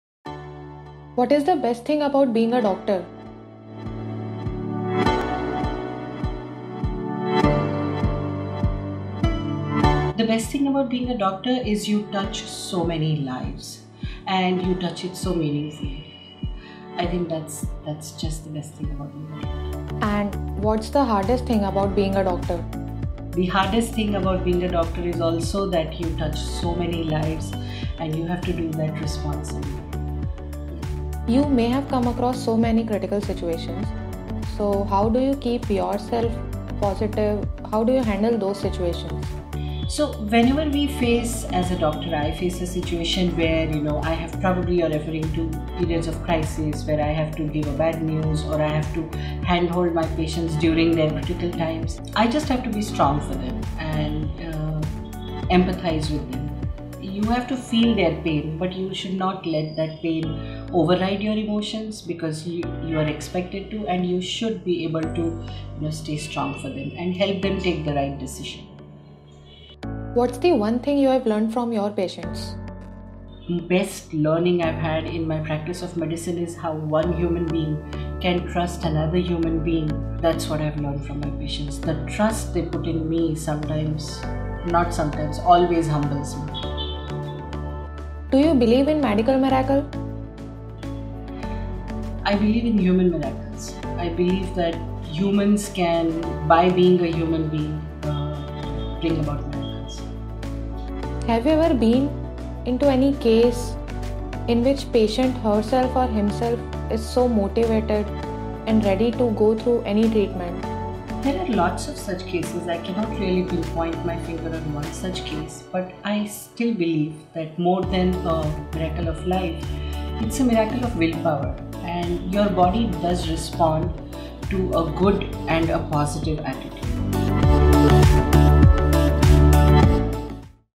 Dr. Anagha Zope | Human First - Episode 2
The highs and lows, motivation and challenges, inspiration and rewards...watch what being a doctor means to Dr. Anagha Zope, in this latest episode of 'Human First'